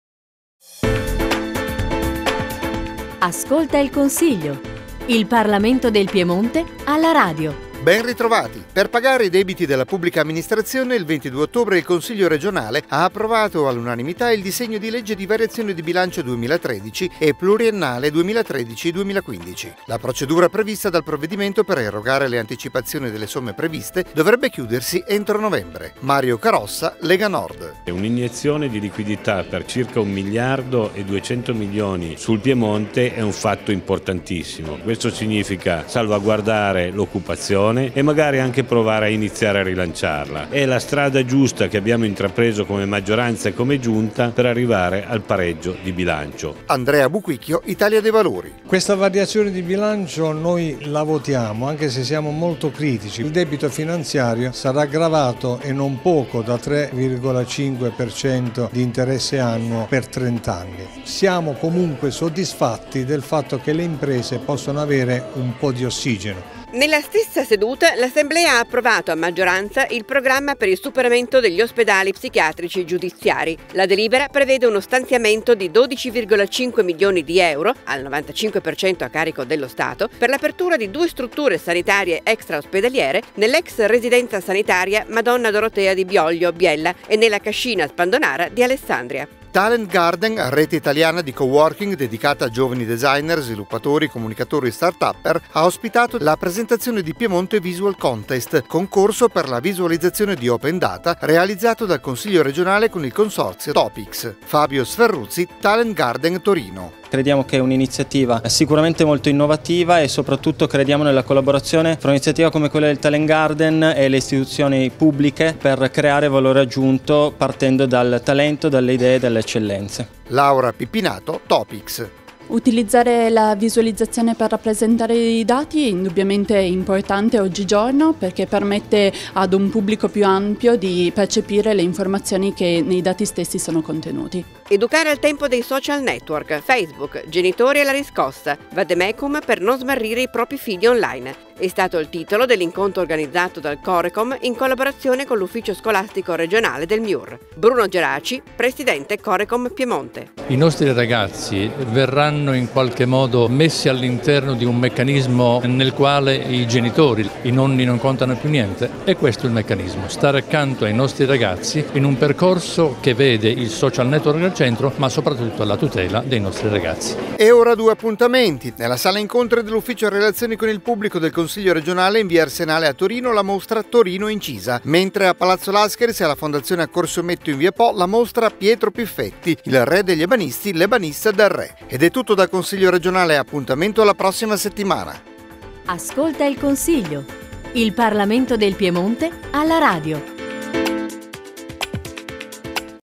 CRP Ascolta il Consiglio n.26 del 24 ottobre 2013
Per pagare i debiti della Pubblica amministrazione il 22 ottobre il Consiglio regionale ha approvato all'unanimità il disegno di legge di variazione di bilancio 2013 e pluriennale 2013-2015. La procedura prevista dal provvedimento per erogare le anticipazioni delle somme previste, dovrebbe chiudersi entro novembre.
Mario Carossa (Lega Nord)
Andrea Buquicchio (Idv)

Nella stessa seduta l'Assemblea ha approvato a maggioranza il programma per il superamento degli Ospedali psichiatrici giudiziari. La delibera prevede uno stanziamento di 12,5 milioni di euro, al 95% a carico dello Stato, per l'apertura di due strutture sanitarie extraospedaliere nell'ex residenza sanitaria Madonna Dorotea di Bioglio (Biella) e nella Cascina Spandonara di Alessandria.

Talent Garden, rete italiana di coworking dedicata a giovani designer, sviluppatori, comunicatori e startupper, ha ospitato nella sede di Torino la presentazione di Piemonte Visual Contest, concorso per la visualizzazione di open data realizzato dal Consiglio regionale con il consorzio top-Ix.
Fabio Sferruzzi, Taleng Garden Torino
Laura Pippinato, Top-Ix

 "Educare al tempo dei Social Network. Facebook: genitori alla riscossa, vademecum per non smarrire i propri figli on line" è stato il titolo dell'incontro organizzato dal Corecom in collaborazione con l'Ufficio Scolastico regionale del Miur.
Bruno Geraci, presidente Corecom Piemonte

Appuntamenti
Nella Sala incontri dell'Ufficio relazioni con il pubblico del Consiglio regionale, in via Arsenale a Torino, la mostra "Torino incisa".
A Palazzo Lascaris e alla Fondazione Accorsi-Ometto in via Po la mostra "Pietro Piffetti. Il re degli ebanisti, l'ebanista del Re".